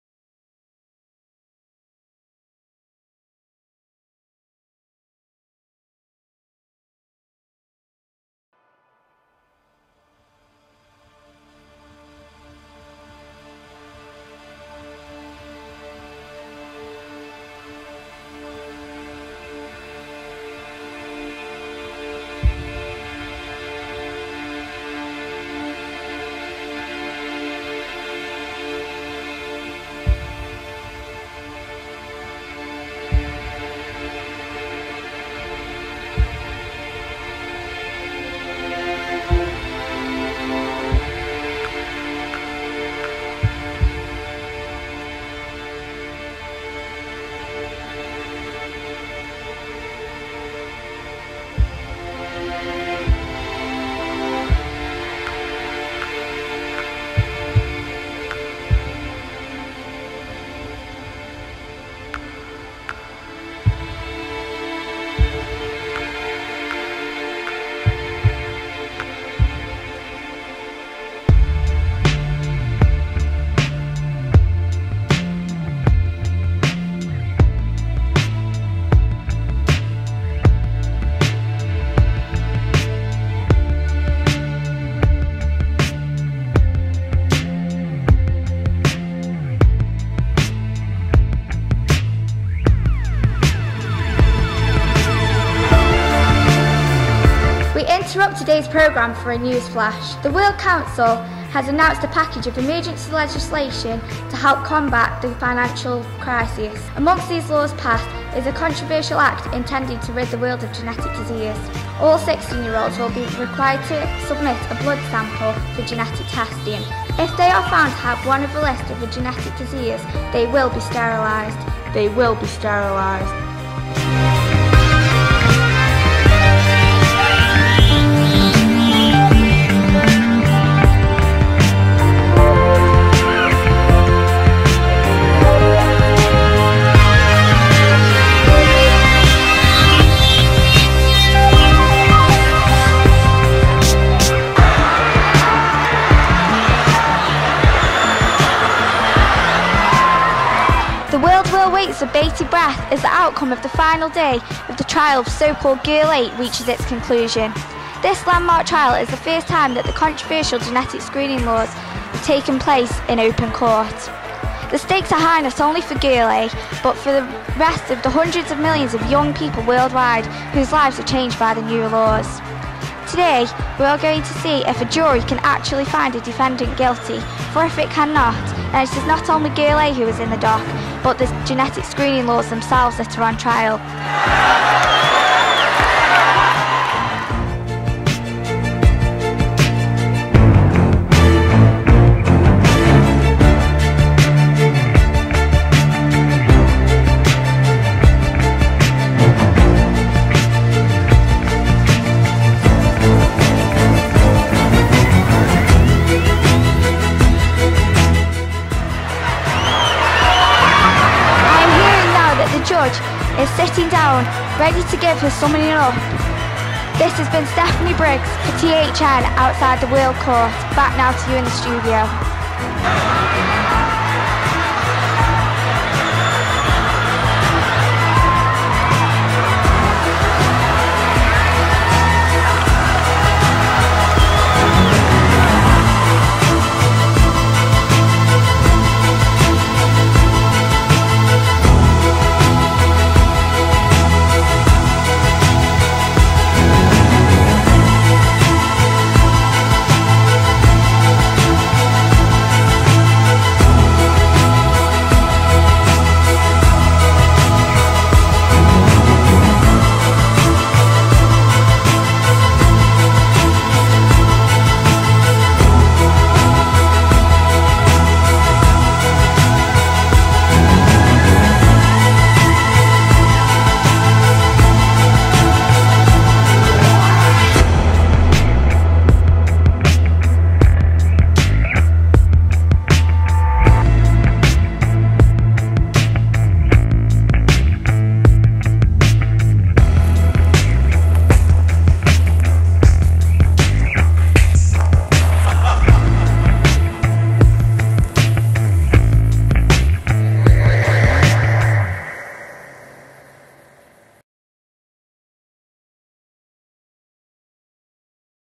Girl A (Trailer)
A Dramatic Enquiry developed by a group of year 9 pupils at Thistley Hough High School in Stoke-on-Trent. The Enquiry was designed for other schools to engage with the Science curriculum.